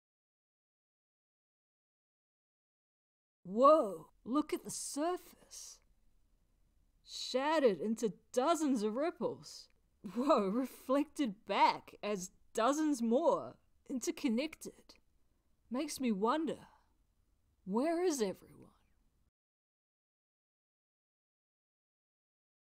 A Talking Well
Unity well that responds to having items thrown in it